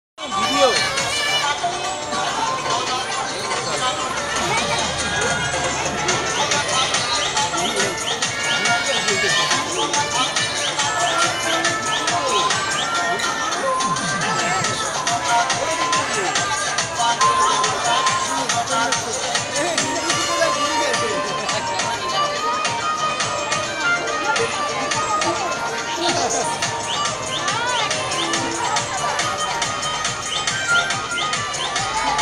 Skeleton Rock Band
Animatronics and lighting on Durga Puja 2017 in Kolkata, India

Might be a bit scary for little kids :)

[VID_20170925_220230]